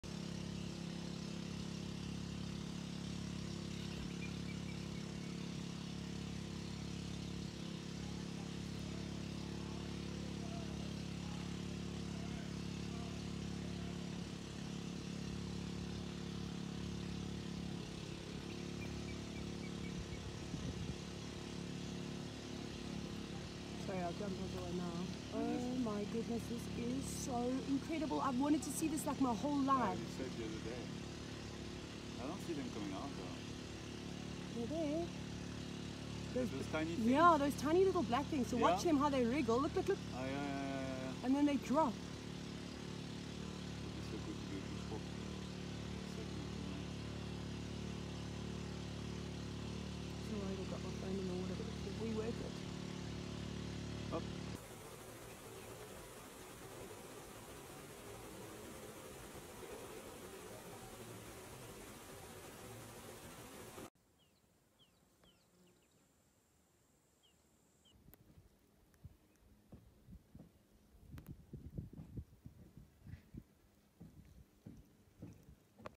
Foam Nest Frogs Hatching | Pridelands Kruger Park
Have you ever wondered how the little tadpoles get out of the adult Southern Foam Nest Frogs nest?
Well, no need to wonder any longer. Instructor Tayla McCurdy managed to spot one of the rarest sightings of her career, Foam Nest Frogs Hatching! Let's take a look!

Follow the EcoTraining journey on social media @EcoTraining or visit our to find out more about the courses we offer.